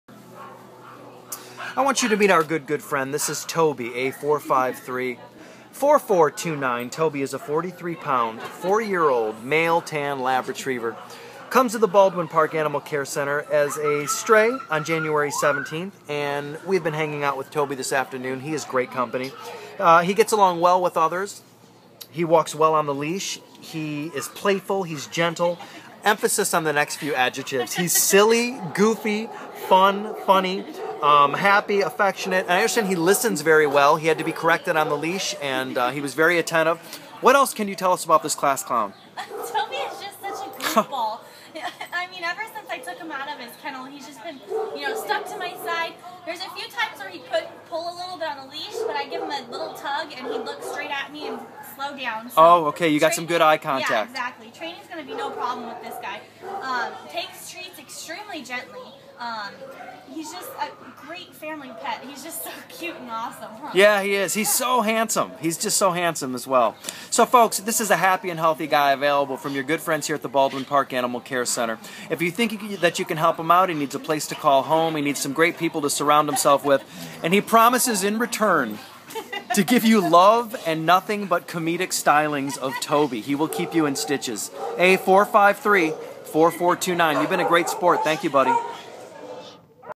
Toby A4534069 ADOPTED!!!!
Toby is a cheerful one year old brown neutered male Pug mix puppy who was found in Monrovia on January 15th and brought to the Baldwin Park Animal Care Center.  Weighing twenty-four pounds, Toby is manageable on leash but could use formal training.  He is good with people of all ages and other dogs of all sizes, and will be a fabulous family dog.  Toby will be a fantastic indoor pet for anyone in any living situation.